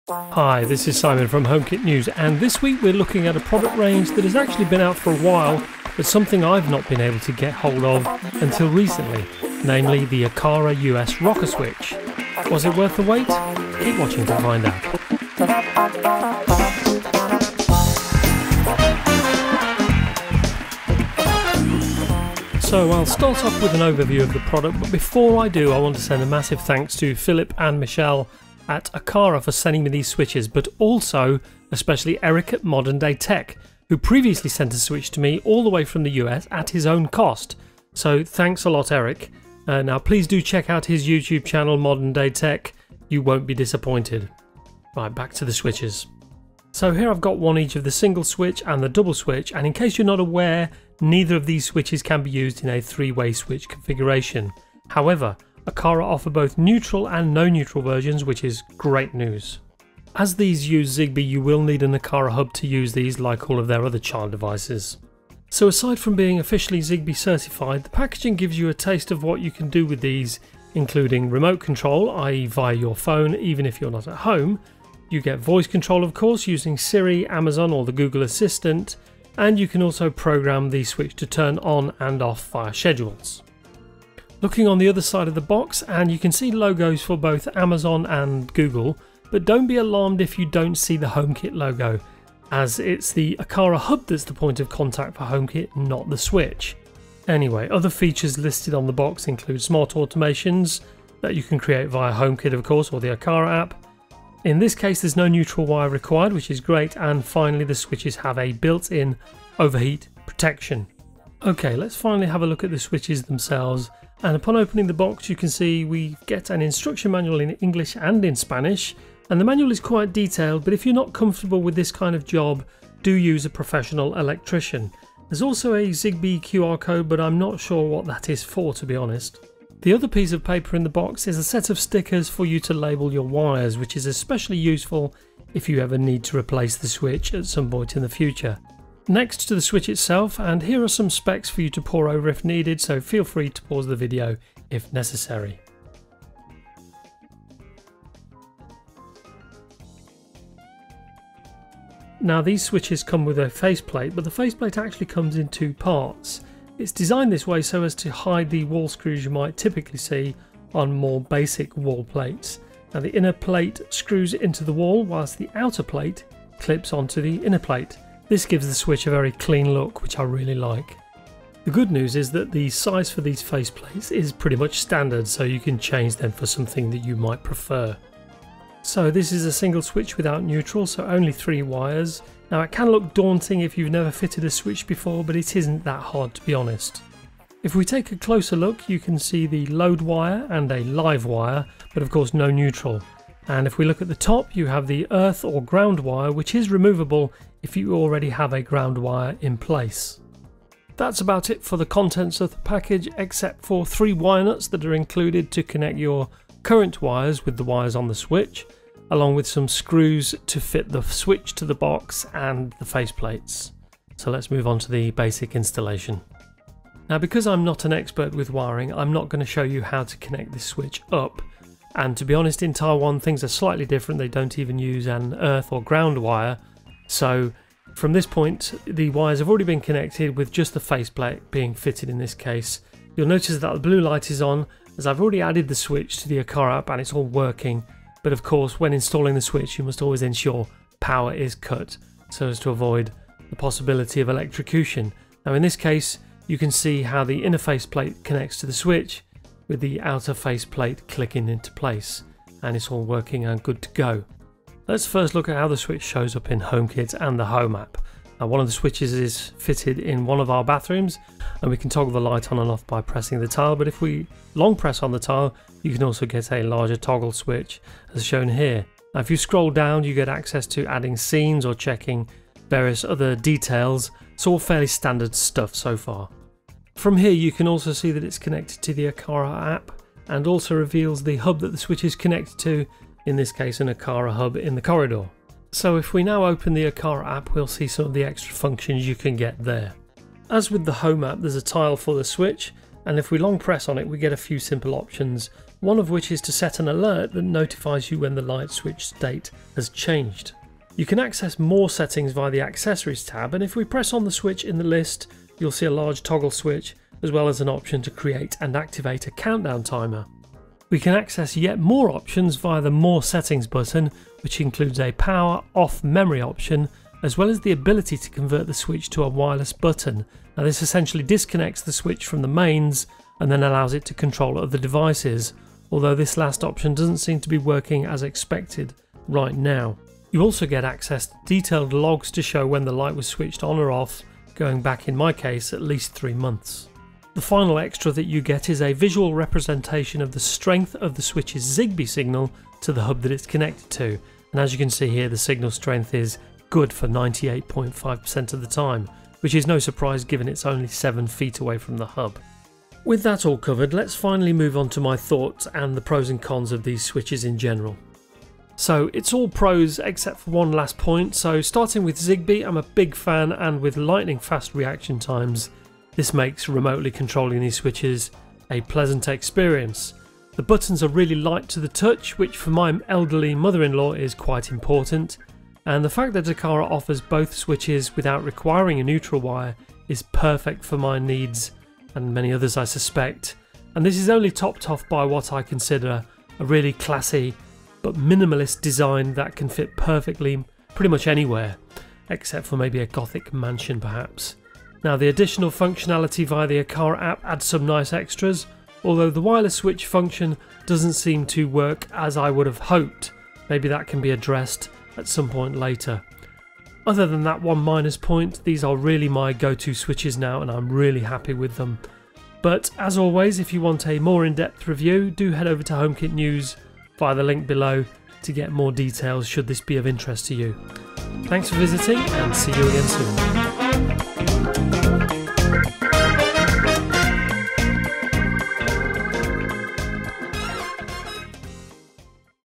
Aqara US Rocker Switch (no neutral) - The Best Budget Option For Smart Switches
An overview of the Aqara Rocker Switches suitable for the US, a switch available in four different options that have actually been around for about a year now. I've only recently been able to get my hands on them though, but it's always good to look on a product that has had time to mature and iron out any potential issues.

To see how Rey G installed his switches, check out the following video -

Single rocker (no neutral)
Double rocker (no neutral)

Single rocker (with neutral)
Double rocker (with neutral)

Timestamps
0:00 intro
0:24 Overview and features
1:52 Unboxing
2:21 Specifications
2:31 The wallplate
3:08 Closer look at the wiring
3:37 Installation (partial)
4:14 App control (Apple Home)
4:49 App control (Aqara Home)
5:58 Pros & cons - outro